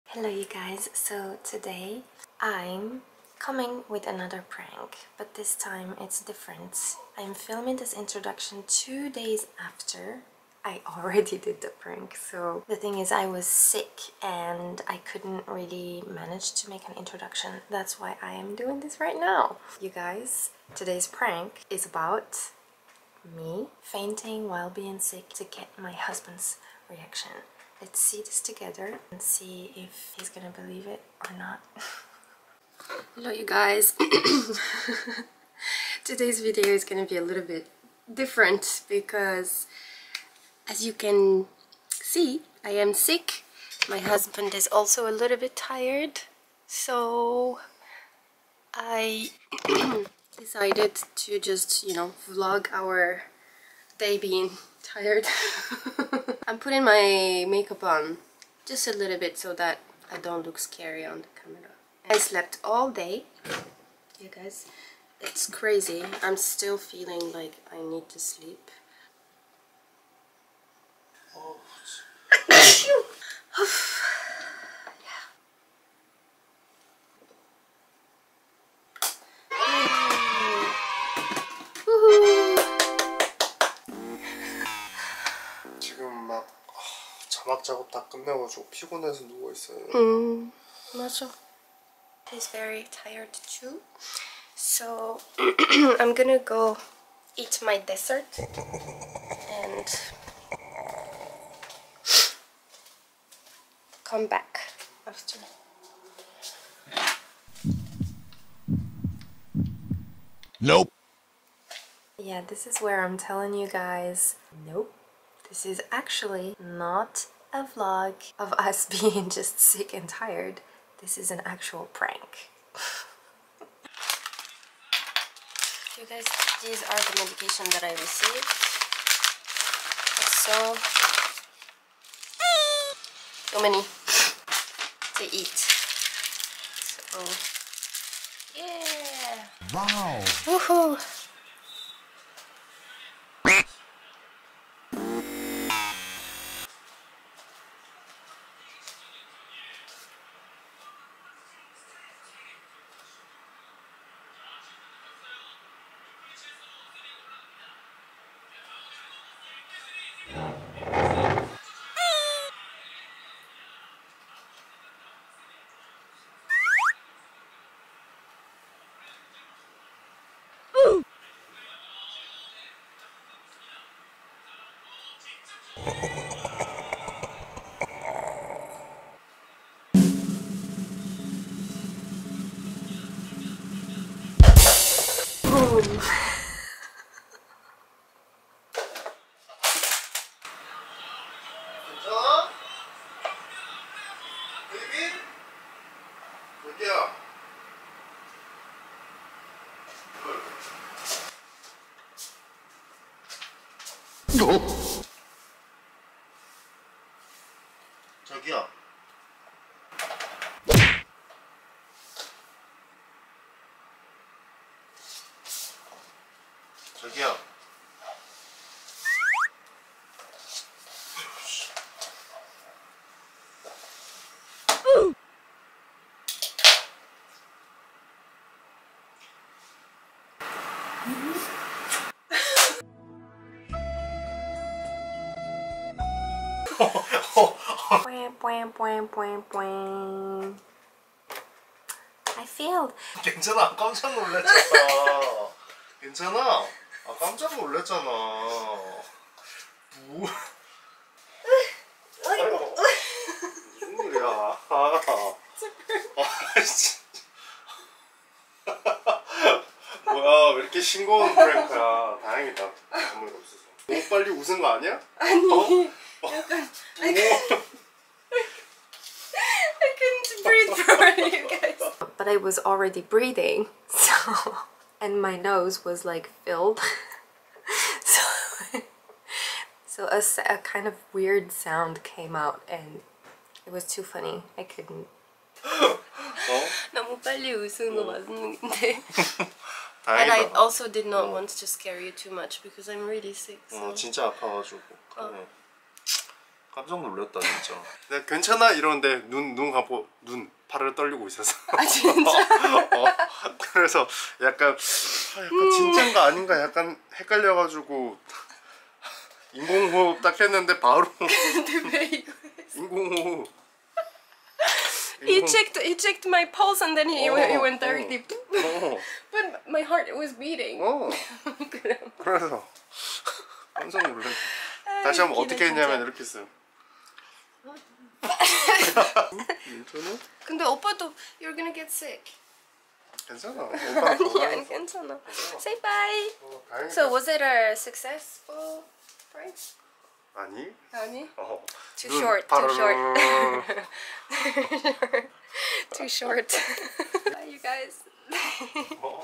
[SUB] Fainted while very sick PRANK on my husband *FAIL* 🤣😓
💖Caption 💖
Hello besties,

I tried my best while being sick but I failed. I had a lot of fun making it and editing. I hope you'll enjoy my epic fail lol.